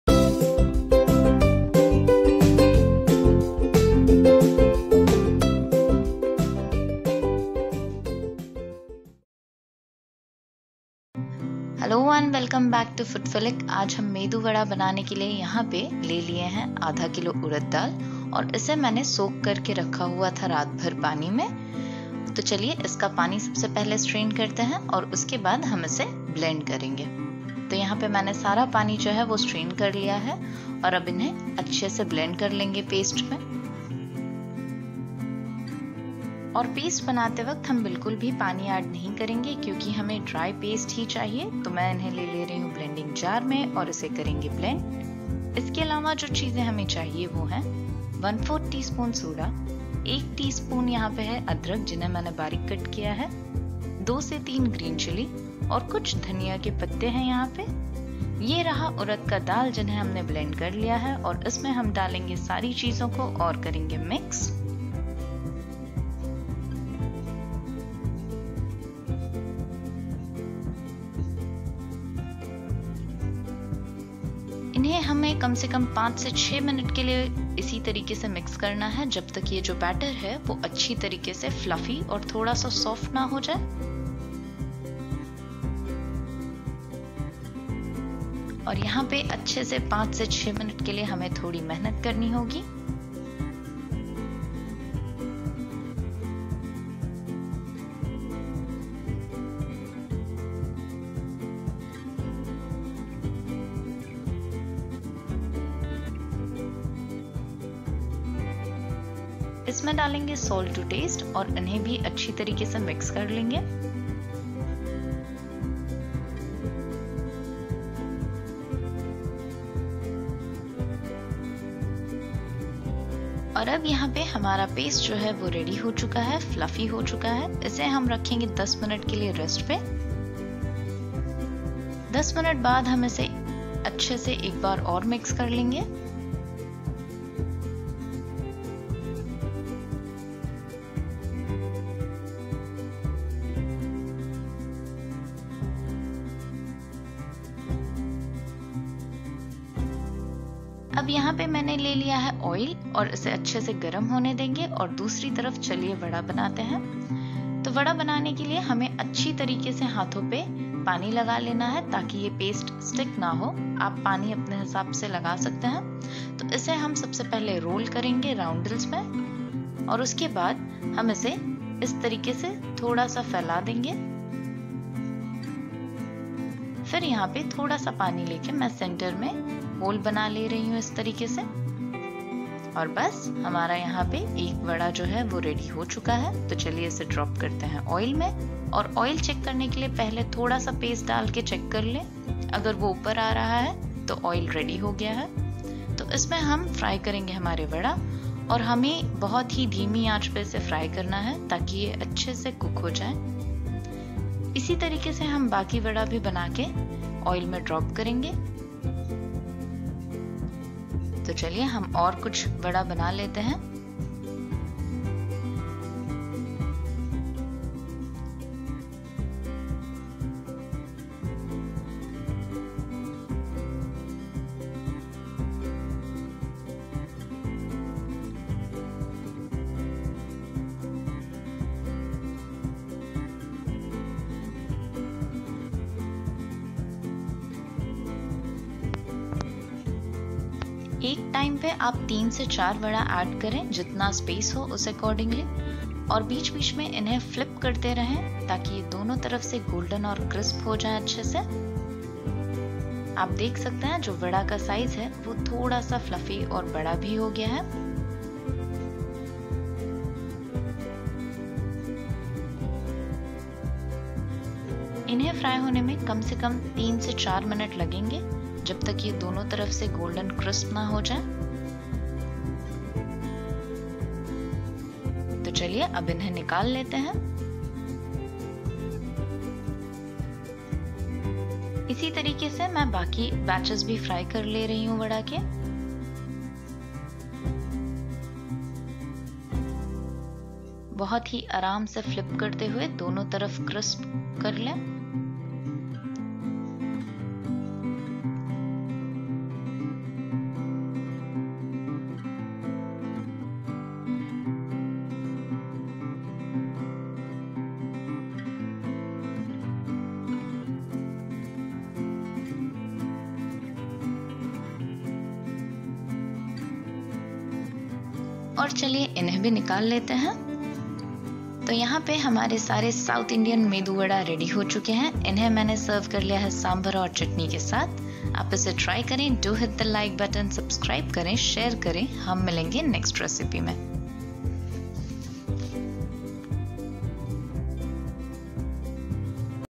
Medu vada recipe || Crispy South Indian medu vada recipe
Crispy and tasty street style Medu vada recipe@ home

Ingredients:
Urad Dal -1/2 kg

Method:
1) Soak urad Dal overnight.
2)Blend soaked dal into smooth paste.
3)Add chopped green chilly and Ginger.
4)Add soda to this and mix well for 5-6 minutes.
5)Now allow the paste to rest for 10 minutes.
6)After 10 minutes add salt to taste and mix well.
7) Shape vada by applying some water on palms .
8)Deep fry the vadas on low flame till it turns golden and crisp.
9) Serve hot with sambhar and chutney.

Enjoy this crispy and tasty breakfast recipe and do let me know how it came out in the comment section below.

Don't forget to press the 🔔 icon to stay updated with new recipes.